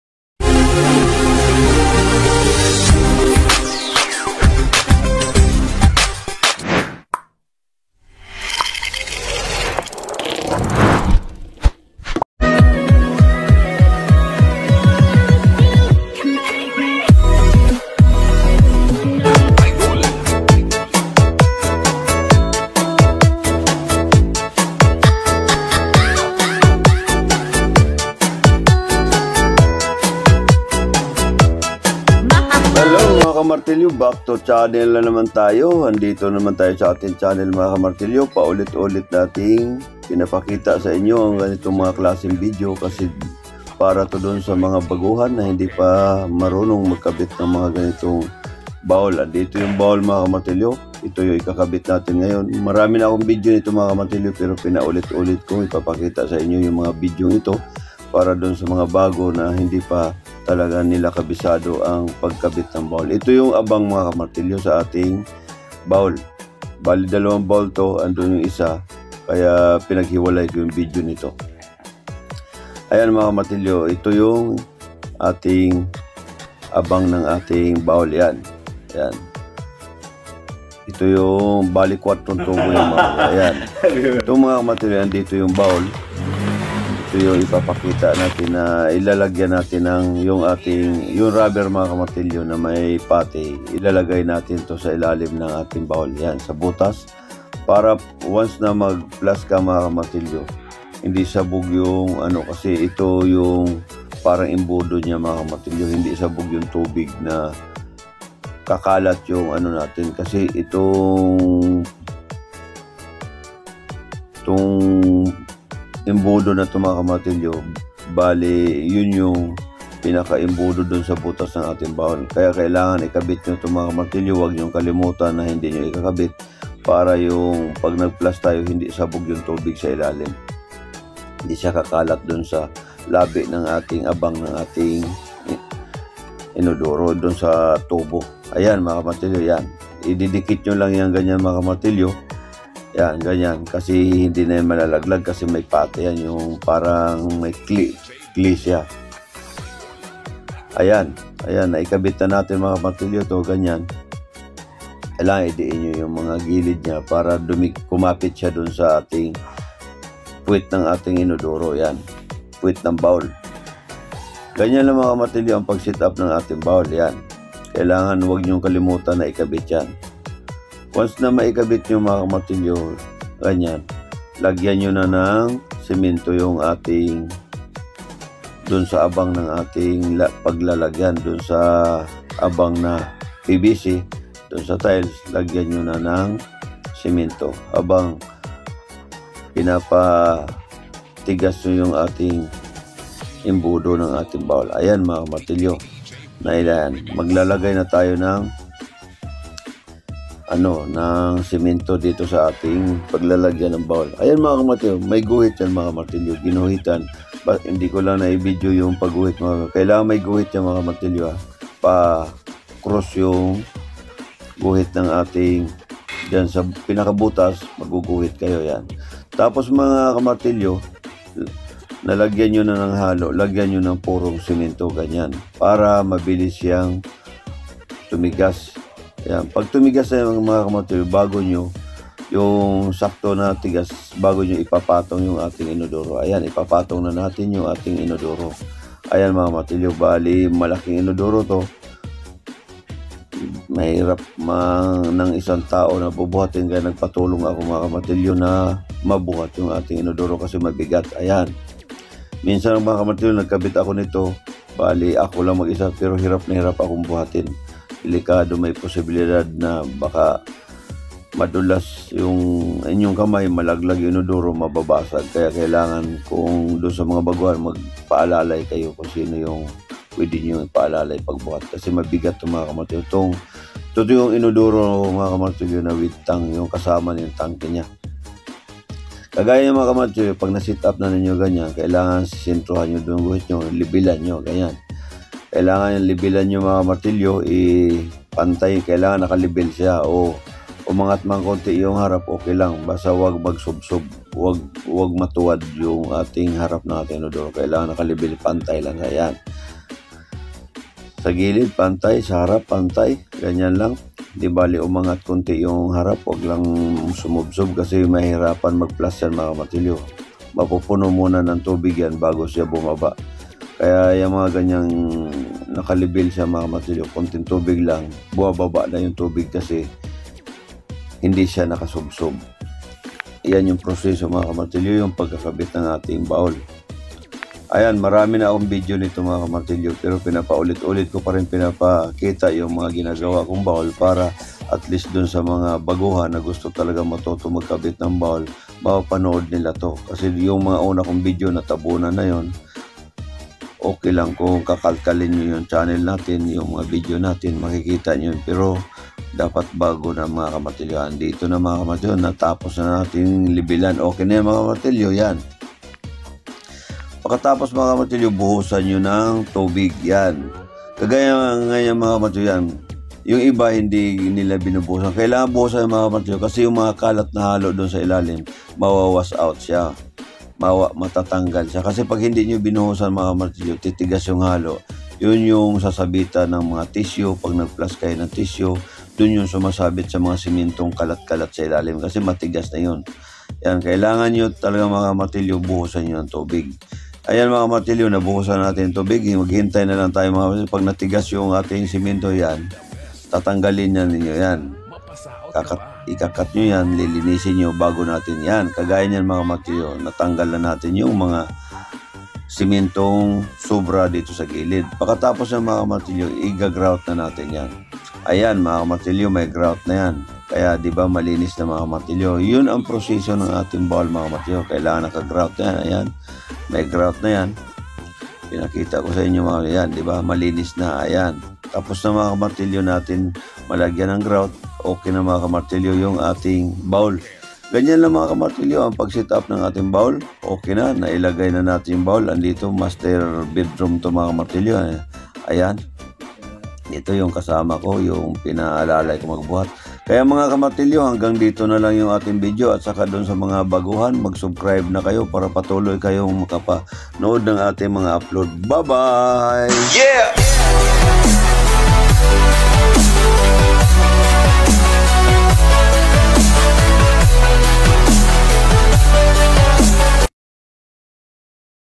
TOILET BOWL Tips Ko Sa Tamang Pag Kabit.
Tips kung paano paano mag abit ng toilet bowl by martilyo lagare